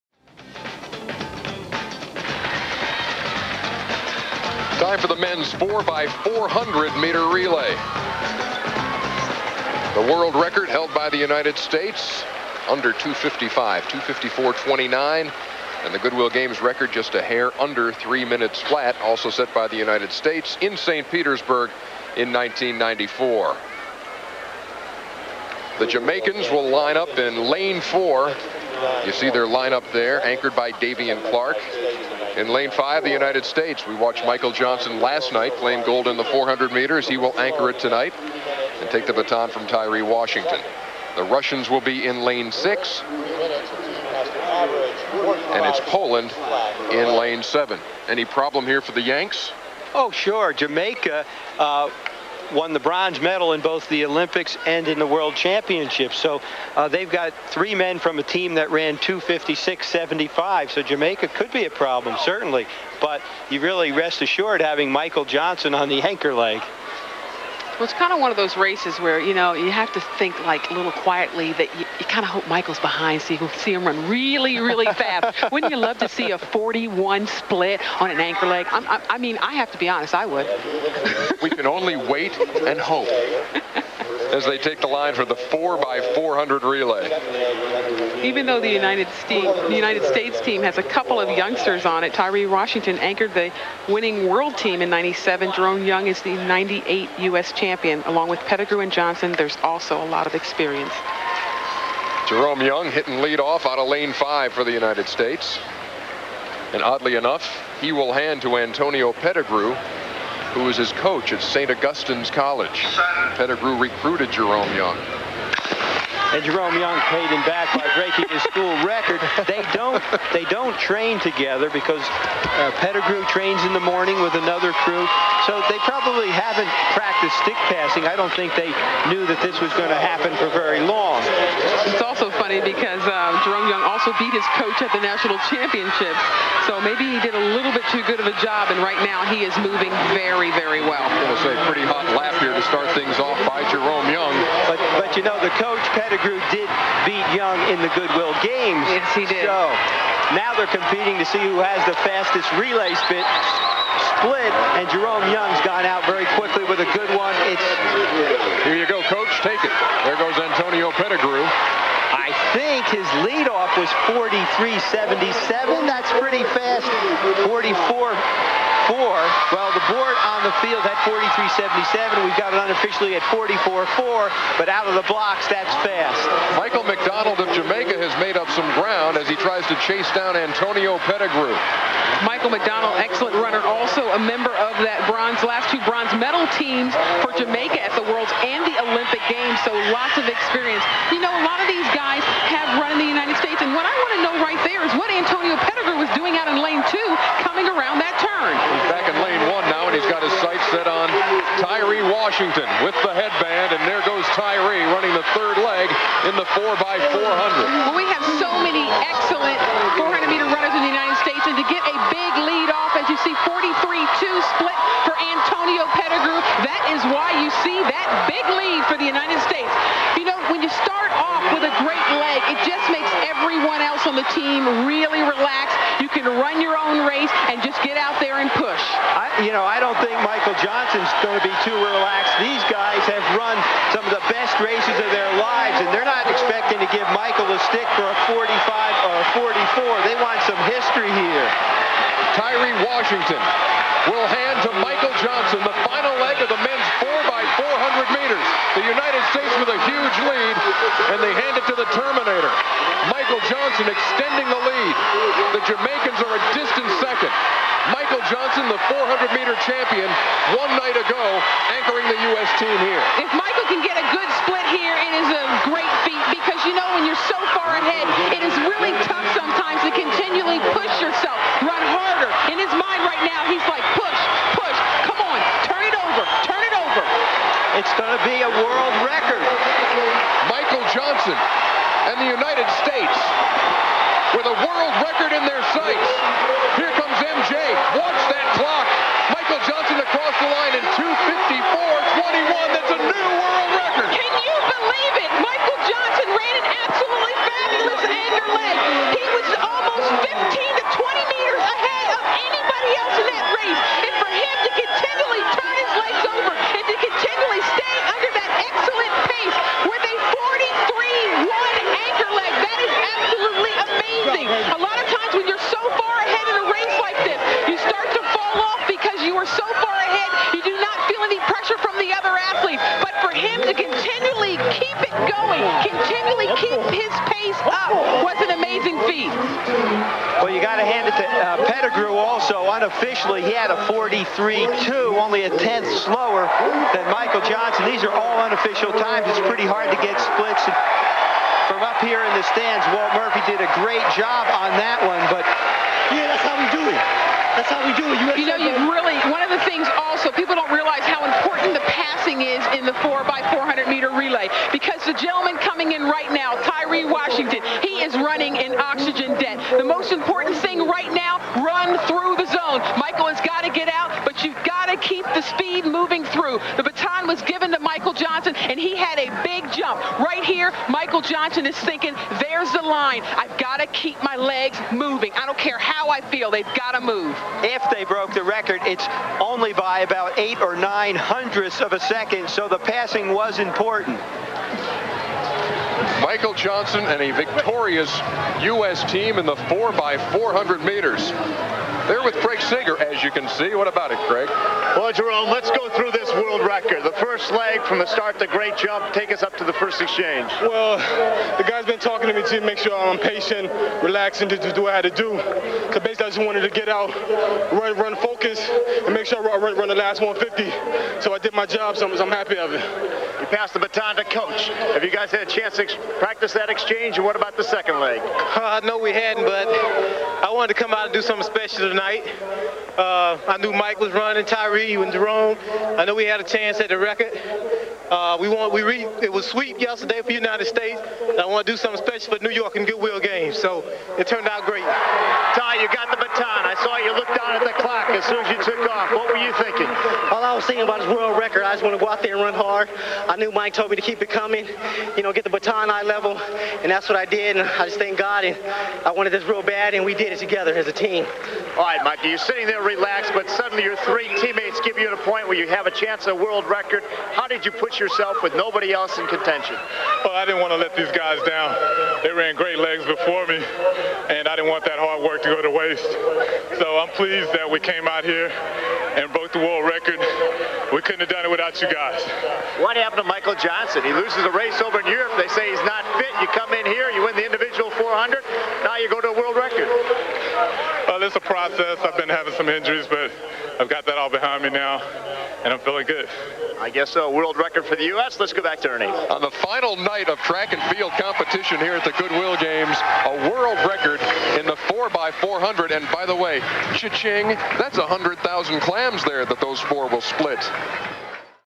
Men's 4 x 400m Relay - 1998 Goodwill Games
Goodwill Games
Uniondale, New York
July 22, 1998
Men’s 4 x 400m Relay Results:

     1) Poland – 2:58.00
Piotr Rysiukiewicz, Tomasz Czubak,
Piotr Haczek, Robert Mackowiak

     2) Jamaica – 2:58.33
Greg Haughton , Michael McDonald,
Michael Blackwood, Davian Clarke

     3) Russia – 3:06.53
Dmitriy Bey, Dmitriy Kosov,
Boris Gorban, Vladislav Shiryayev

      –  United States – 2:54.20 (DQ on 8/12/08)
Jerome Young, Antonio Pettigrew,
Tyree Washington, Michael Johnson